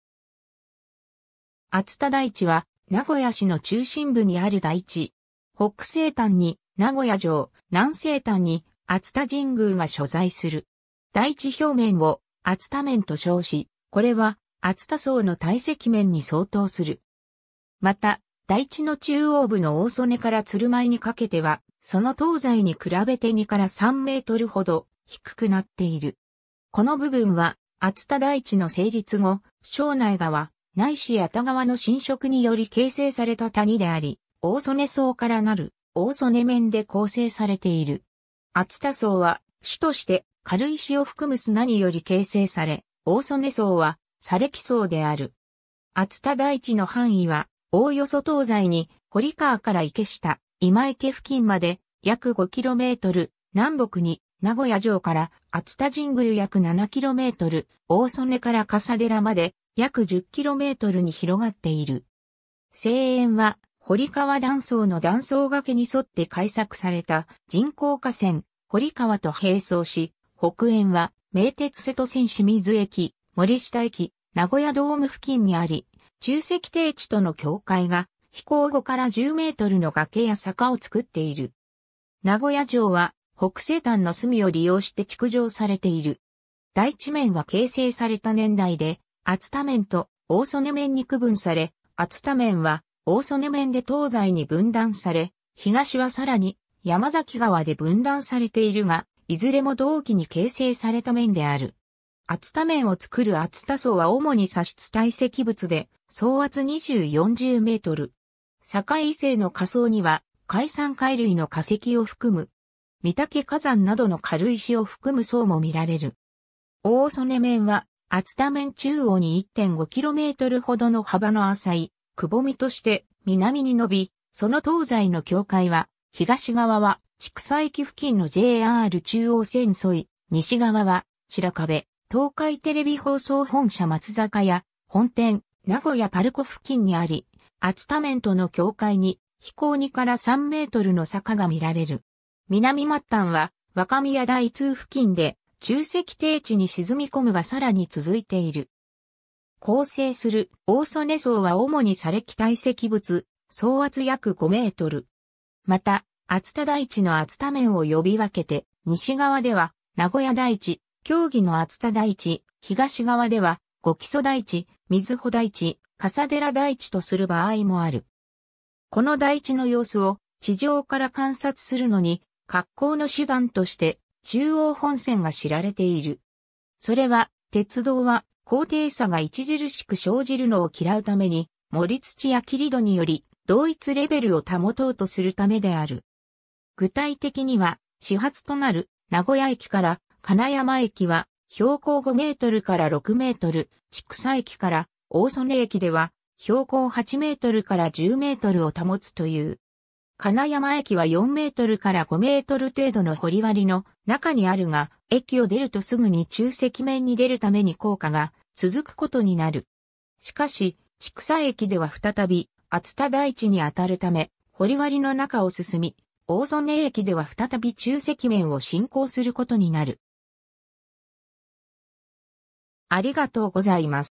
熱田台地
熱田台地（あつただいち）は、名古屋市の中心部にある台地。
 北西端に名古屋城、南西端に熱田神宮が所在する。
台地表面を熱田面と称し、これは熱田層の堆積面に相当する。
また、台地の中央部の大曽根から鶴舞にかけては、その東西に比べて2 - 3メートルほど低くなっている。
この部分は熱田台地の成立後、庄内川ないし矢田川の浸食により形成された谷であり、大曽根層から成る大曽根面で構成されている。
熱田層は主として軽石を含む砂により形成され、大曽根層は砂礫層である。
 熱田台地の範囲は、おおよそ東西に堀川から池下・今池付近まで約5km、南北に名古屋城から熱田神宮約7km、大曽根から笠寺まで約10kmに広がっている。
西縁は堀川断層の断層崖に沿って開削された人工河川・堀川と並走し、北縁は名鉄瀬戸線清水駅 - 森下駅 - ナゴヤドーム付近にあり、沖積低地との境界が比高5-10メートルの崖や坂を作っている。
名古屋城は北西端の隅を利用して築城されている。
 台地面は形成された年代で熱田面と大曽根面に区分され、熱田面は大曽根面で東西に分断され、東はさらに山崎川で分断されているが、いずれも同期に形成された面である。
 熱田面を作る熱田層は主に砂質堆積物で層厚20−40m。
栄以西の下層には海産貝類の化石を含む。
御岳火山などの軽石を含む層も見られる。
 大曽根面は熱田面中央に1.5kmほどの幅の浅い窪みとして南に伸び、その東西の境界は、東側は千種駅付近のJR中央線沿い、西側は白壁 - 東海テレビ放送本社 - 松坂屋本店・名古屋パルコ付近にあり、熱田面との境界に比高2-3mの坂がみられる。
南末端は若宮大通付近で沖積低地に沈み込むが更に続いている。
構成する大曽根層は主に砂礫堆積物、層厚約5m。
 また熱田台地の熱田面を呼び分けて、西側では、那古野台地（北部、城・中心街）、狭義の熱田台地（南部、旧熱田の象の鼻状部）、東側では御器所台地（北部）、瑞穂台地（南部）、笠寺台地（山崎川以南）とする場合もある。
 この台地の様子を地上から観察するのに格好の手段として、中央本線が知られている。
それは鉄道は高低差が著しく生じるのを嫌うために、盛土や切土により、同一レベルを保とうとするためである。
具体的には始発となる名古屋駅から金山駅は標高5メートルから6メートル、千種駅から大曽根駅では標高8メートルから10メートルを保つという。
金山駅は4メートルから5メートル程度の堀割の中にあるが、駅を出るとすぐに沖積面に出るために高架が続くことになる。
しかし、千種駅では再び熱田台地にあたるため、堀割の中を進み、大曽根駅では再び沖積面を進行することになる。